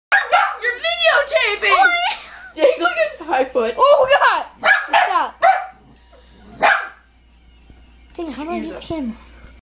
I pod accident
He was trying to take a pic